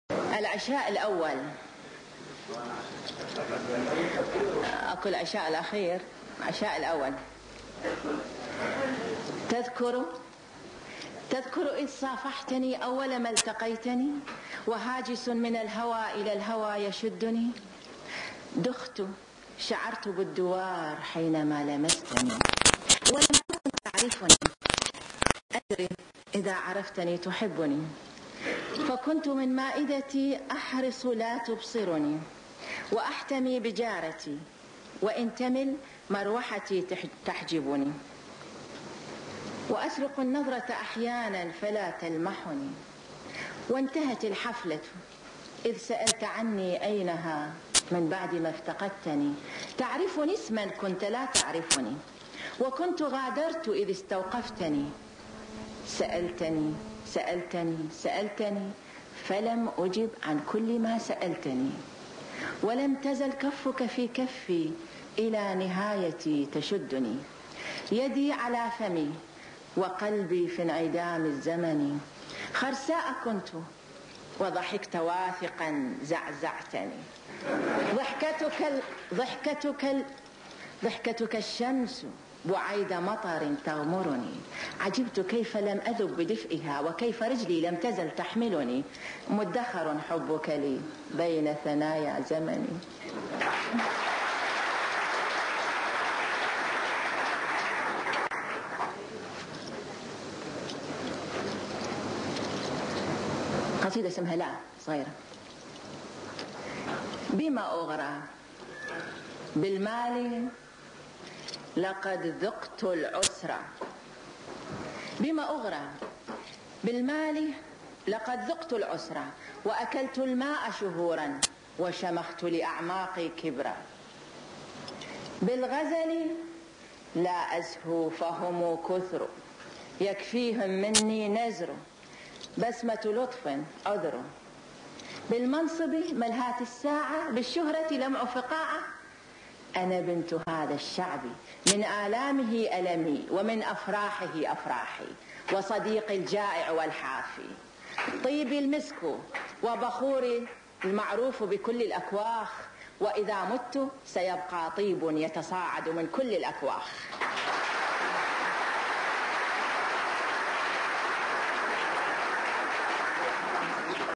لميعة عباس عمارة - قصيدة العشاء الاول وقصيدة لا
شاعرة عراقية كبيرة فهي سومرية السلالة ..وحين ولدت في منطقة الشواكة من كرخ بغداد لاحت في عينيها الواسعتين ظلالاً لزقوّرة أور ودارت فيها حكايا وأساطير .. وحين عمّدوها تلك المندائية الصغيرة بنهر العمارة ما كانوا يعلمون بأنها ستسرق من الماء عذوبته ورقته .. وعندما أزهرت كشابة أتسعت عيناها أكثر فبدت كشبعاد وأخذ قوامها شكل وأنحناءات قوام عشتار